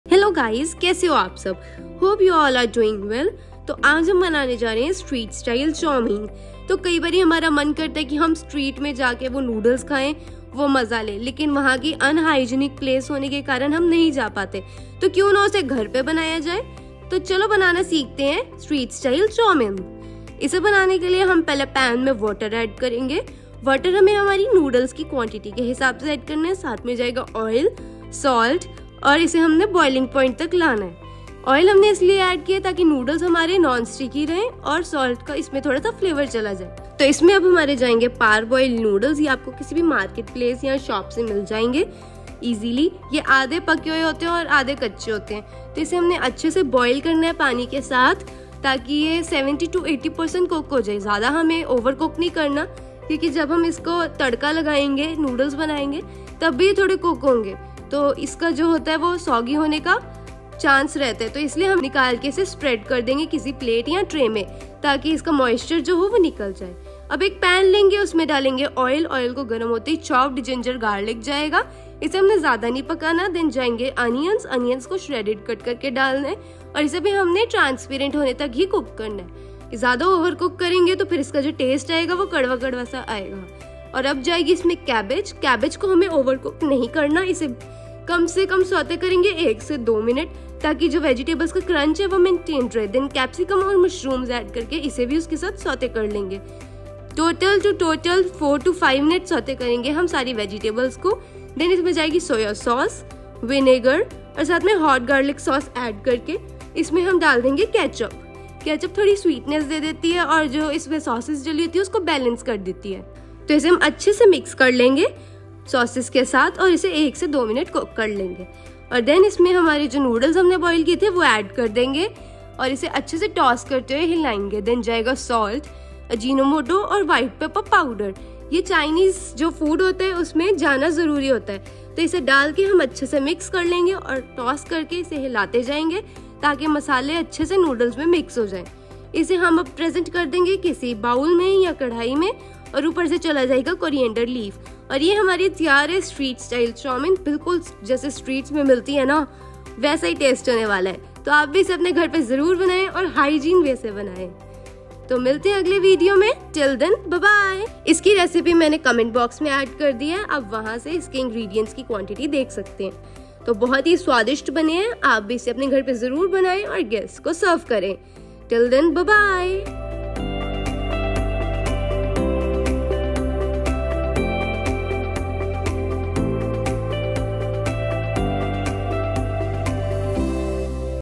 Street Style Chowmein | chowmein recipe | Chowmein recipe in hindi
Strreet Style Chowmein and chowmein recipe in hindi by Cheshtha Anand.
chowmein recipe, chaumeen, how to make chowmein